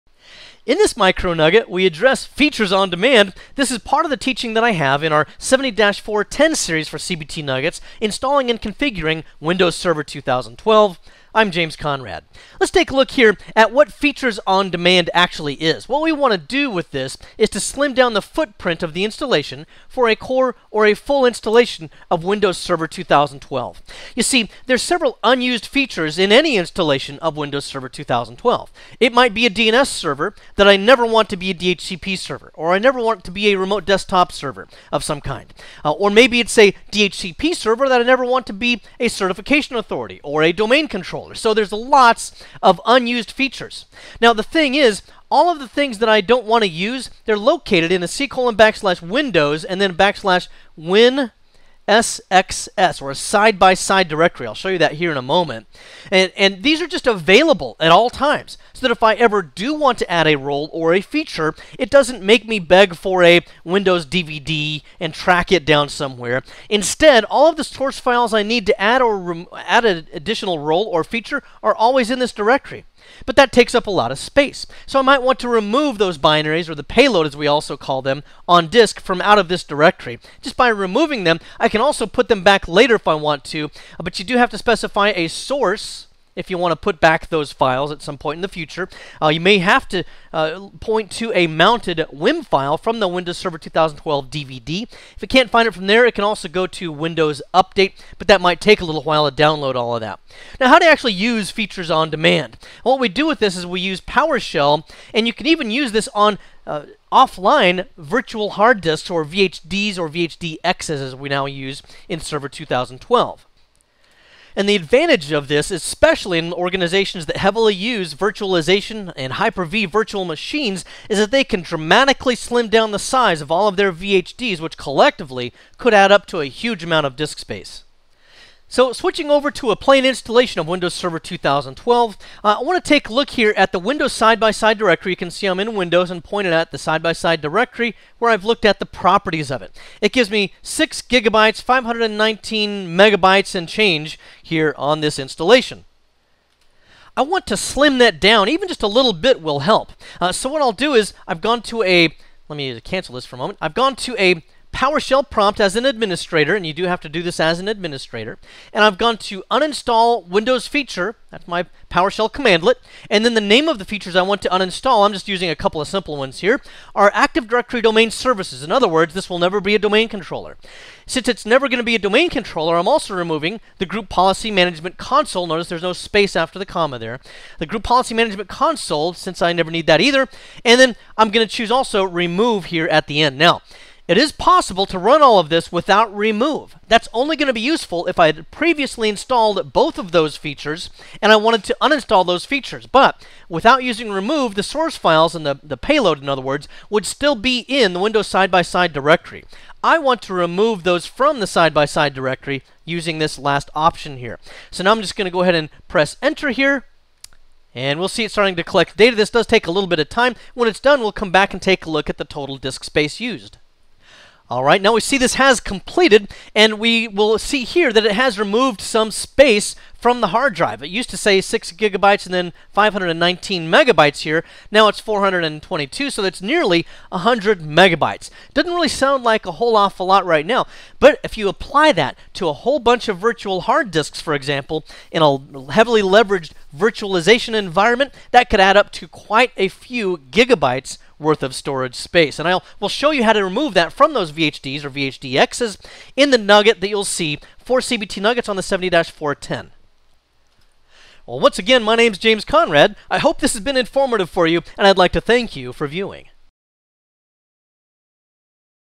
MicroNugget: How to Reduce the Server 2012 Installation Footprint
In this video, James Conrad covers reducing the installation footprint of Windows Server 2012. Whether on a core or a full installation of Windows Server 2012, there will always be several unused features in any installation. Cleaning them up can free up a lot of disk space.

For organizations that are heavily invested in virtualization or use a lot of hyper-v virtual machines, the amount of bloat that comes along with unused features of Windows Server 2012 can result in a lot of disk space going to waste. The features aren't unused in error — they might be things like a DNS server that we know in advance we'll have no reason to ever use as a DHCP server or a remote desktop server, or maybe we have a DHCP server that we don't ever want to be a Certification Authority or Domain Controller.

Windows Server 2012 stores all these features in the same directory, which makes finding them easy, but it also makes removing them a breeze when you're looking to free up a lot of that virtual hard disk (VHD/VHX) space.